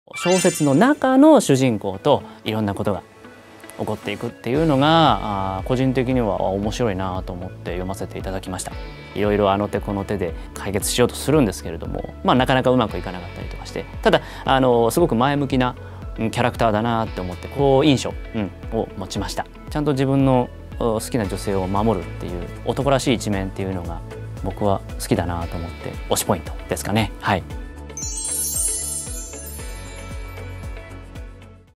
【声優インタビュー】『主役の初体験、私が奪っちゃいました』ゼロニスを演じた河西健吾さんが語ります！
本編動画はこちらから！

概要欄から応募で河西健吾さんのサイン入りチェキが当たります！
キャンペーン〆切：2022年2月10日(木)23:59